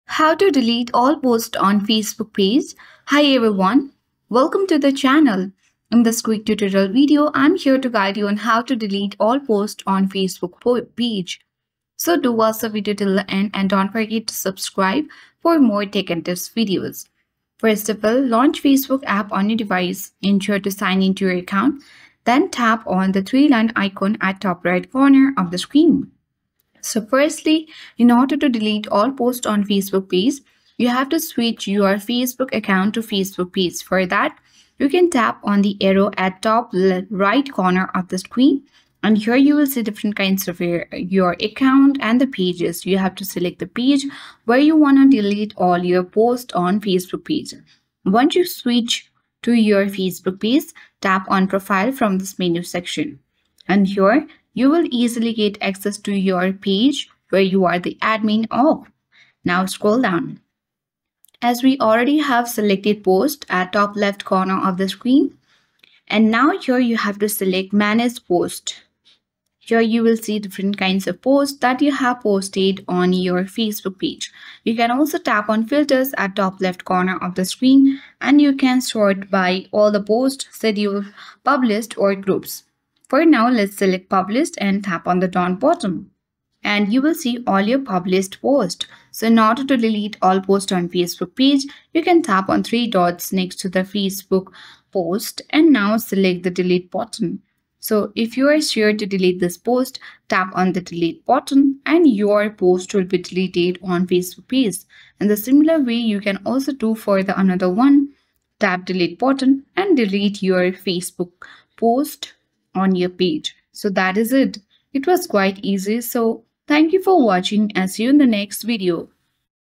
How to Delete All Posts on Facebook Page
Learn how to delete all posts on a Facebook page with this easy-to-follow tutorial. In just 2 minutes, you'll master the steps to manage and remove posts from your Facebook page efficiently. This guide is perfect for page admins looking to clean up their content quickly.

Chapters
00:00 - Introduction
Learn how to delete all posts on a Facebook page efficiently.

00:10 - Launch Facebook App
Open the Facebook app and sign into your account.

00:20 - Switch to Facebook Page
Change your view to the Facebook page you manage.

00:35 - Access Page Profile
Tap on "Profile" to access your Facebook page.

00:50 - Navigate to Manage Posts
Select "Manage Posts" to view all your posts.

01:05 - Filter and Select Posts
Filter posts by published status and select the ones to delete.

01:20 - Delete Posts
Tap on the three dots next to a post and choose "Delete."

01:40 - Confirm Deletion
Confirm the deletion of your selected posts.

01:55 - Conclusion